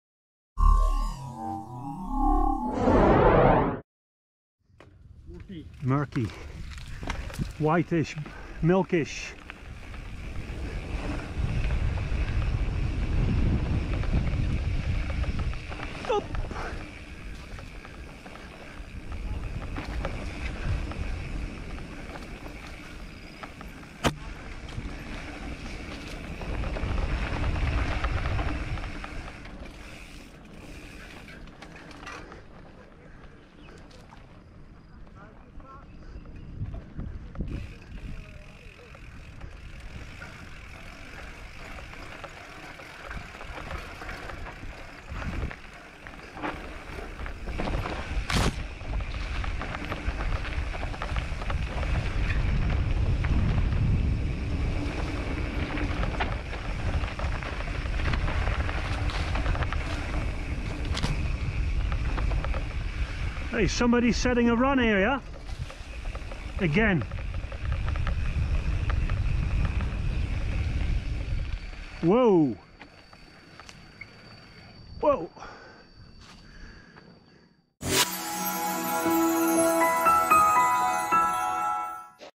Fun | Cruising Along A Ridge In Hambalang | Hambalang, BOGOR
This short video is to give you an impression of riding, experiencing great views, local traditions and encountering funny moments in the mountains, valleys and surroundings of Hambalang in Sentul, INDONESIA.

Many to follow! Exciting "bike along" or "follow cam" episodes from around Indonesia for YOU!